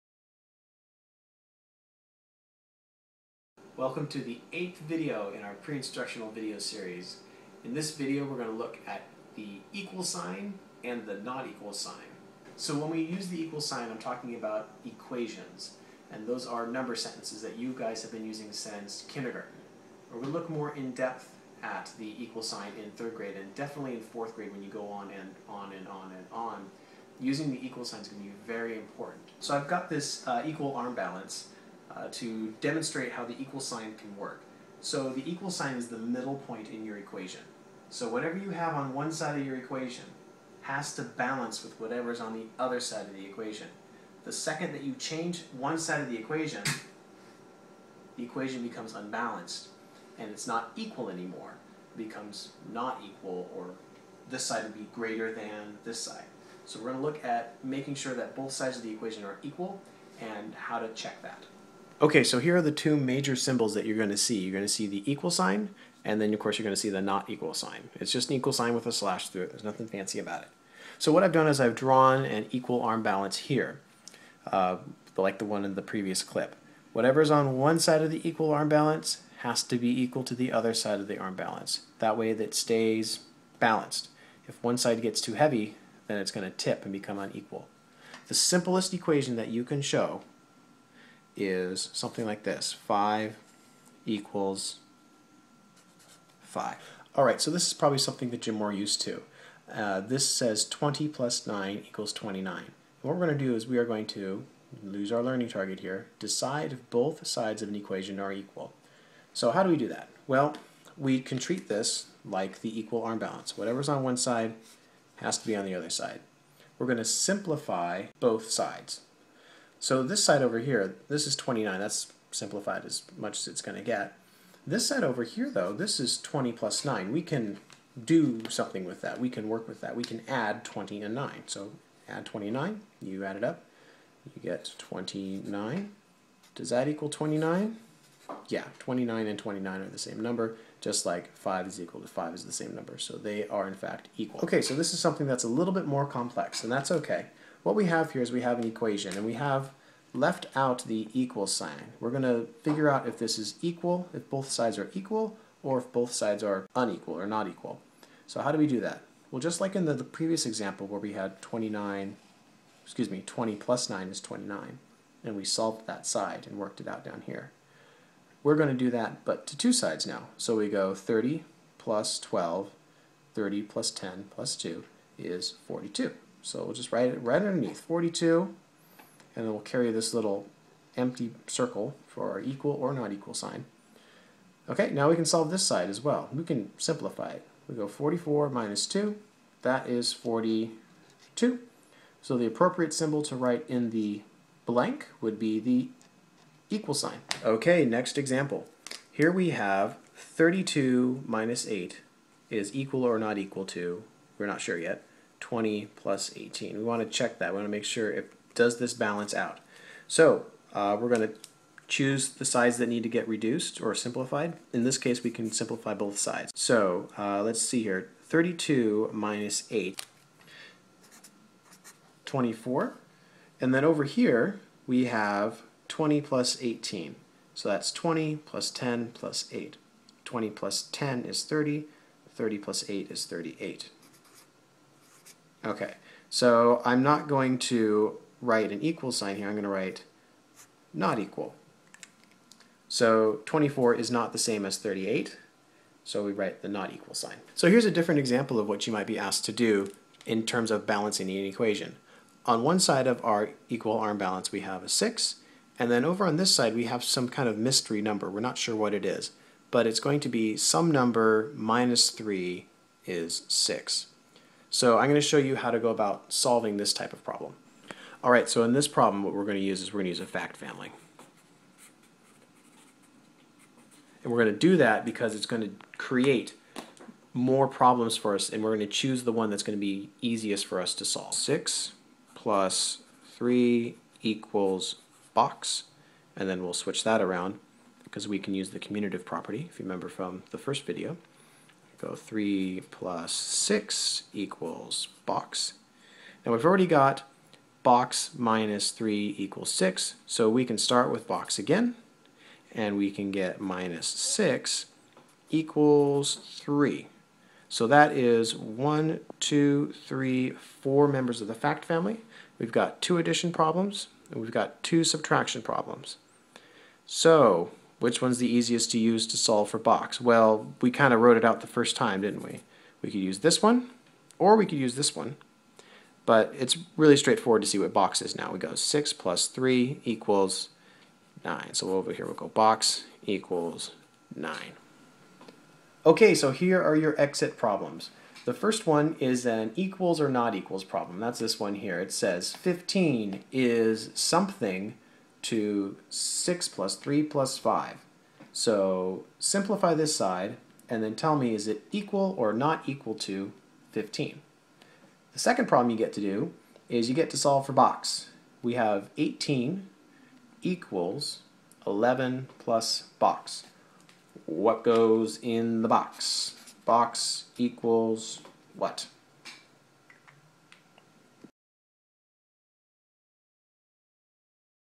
Lesson 2-8 Balanced Equations
3rd grade is where students start to use the structure of the sentence, or equation, to enhance their mathematical power.  They become more flexible in terms of using operation signs on both sides of an equation, rather than simply believing that the expression with the operation goes on the left and the right side is the "answer".

CCSS 3.NBT.2 Fluently add and subtract within 1000 using strategies and algorithms based on place value, properties of operations, and/or the relationship between addition and subtraction.